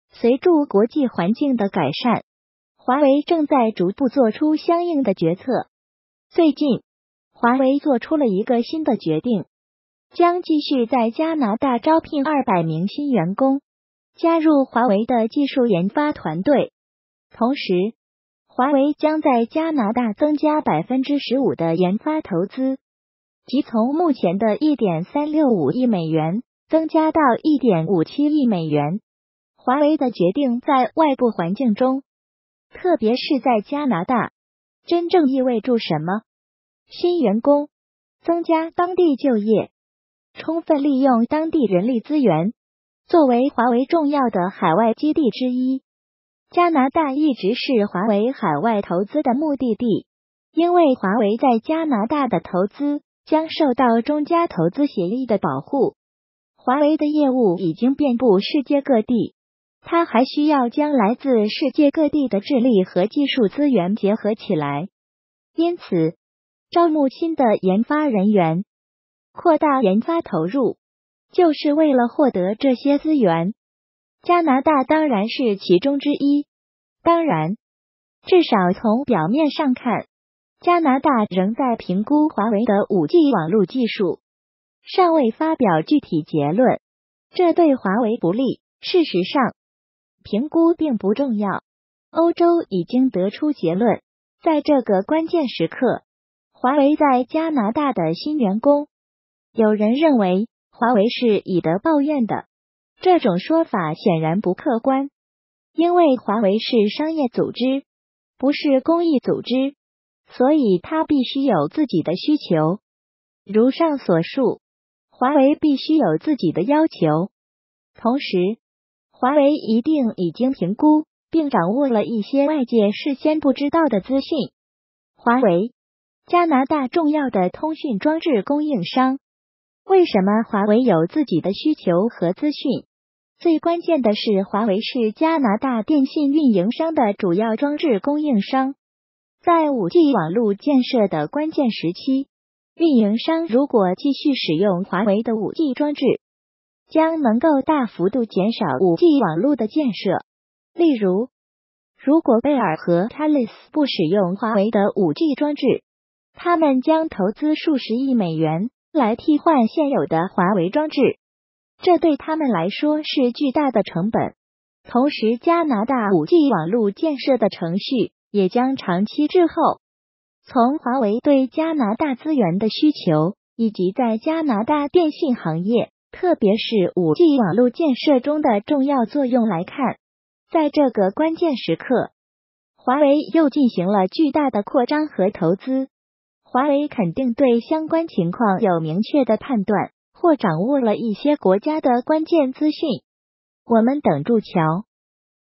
華為的這一項決定，說明加拿大對於華為的態度已經確定
最近，華為做出了一個新的決定：將繼續在加拿大招聘200名新員工。加入華為的技術研發團隊；同時，華為將在加拿大增加15%的研發投資，即從目前的1.365億美元增加到1.57億美元。